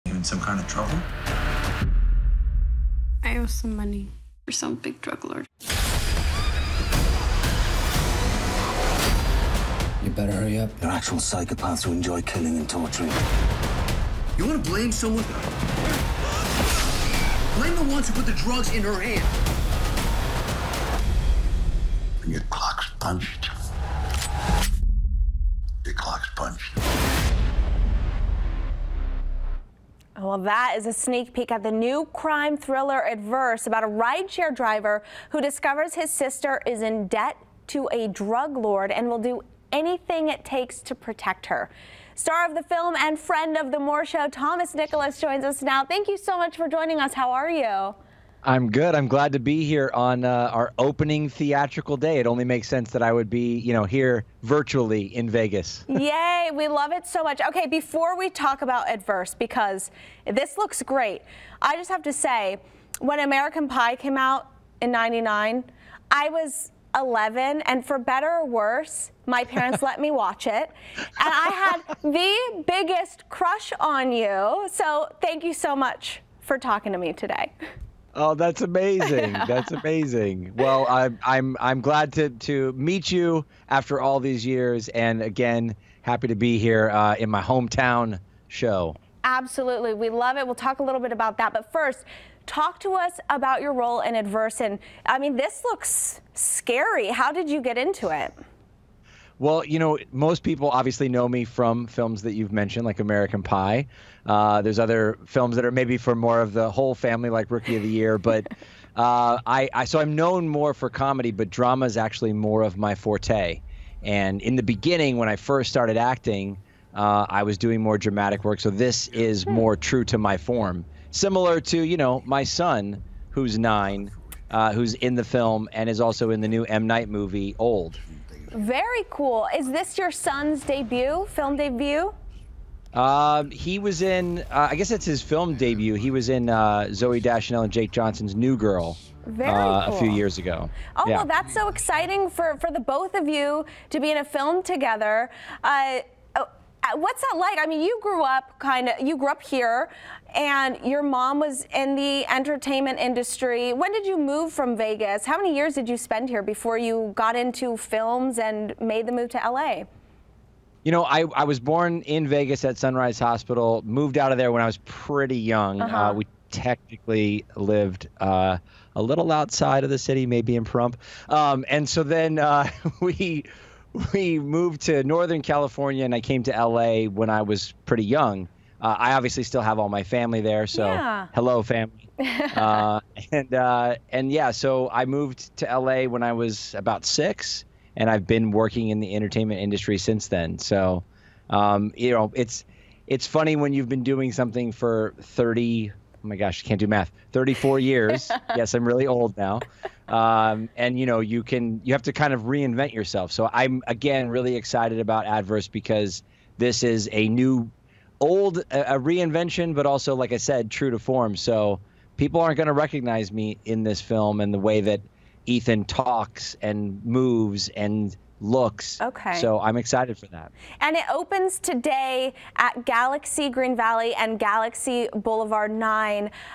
Vegas actor Thomas Nicholas stars in new film, "Adverse"
Vegas actor Thomas Nicholas talks about his role in the new film, "Adverse" now showing at Galaxy Theatres Green Valley & Galaxy Boulevard 9.